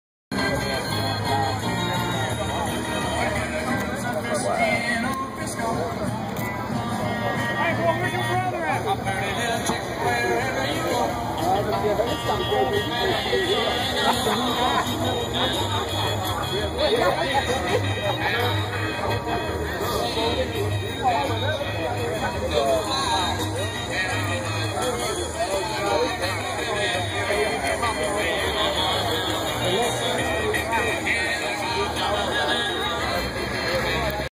Wheels on the water Fayetteville North Carolina
Wheels on the Water a rockabilly style cruise in. Third Friday of the month